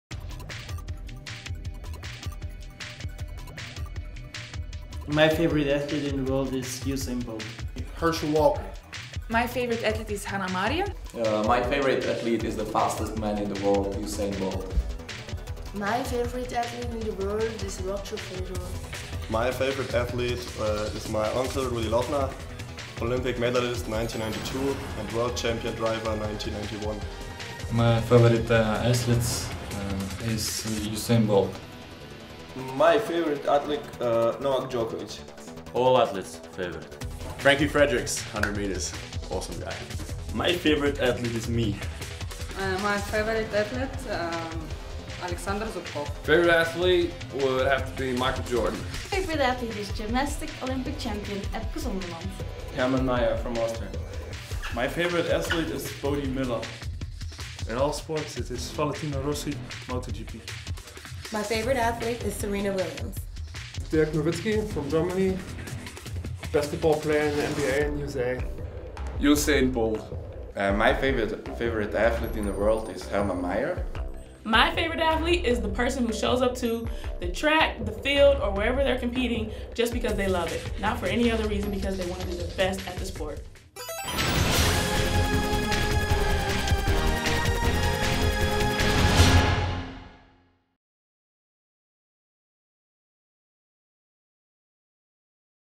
Favorite sportsmen of bobsleigh athletes | FIBT Official
From Usain Bolt to Novak Djokovic, from Frankie Fredericks to Serena Williams. Which is the favorite sportsman according to the bobsleigh athletes? Check it out!